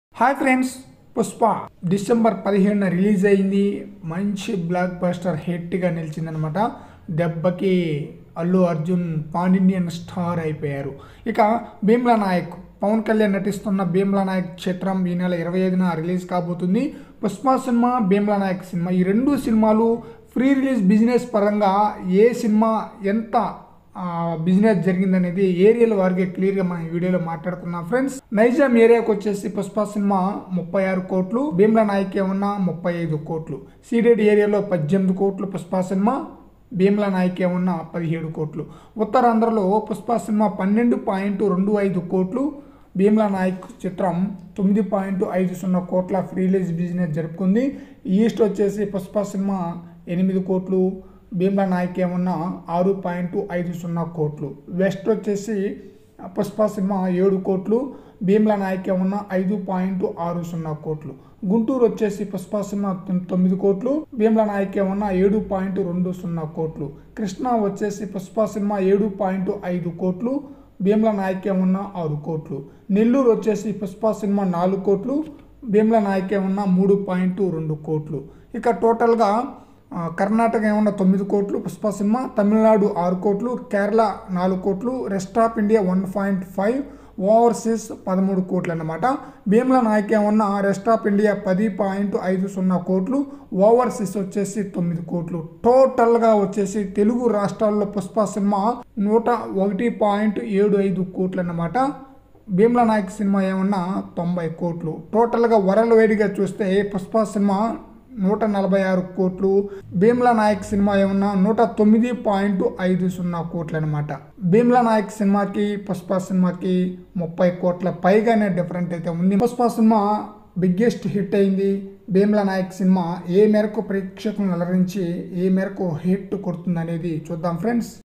భీమ్లా నాయక్ vs పుష్ప ప్రీ రిలీజ్ బిజినెస్ టాప్ ఎవరు? | Bheemla Nayak | KR Films
bheemla nayak movie february 25na release avutundi,allu arjun natinchina pushpa cinema pan india moviega release ayindi,pawan kalyan natistunna bheemla nayak movie ki pushpa cinema movieki pre release business ye cinema yenta jarupukundi anedi ee videolo chudam,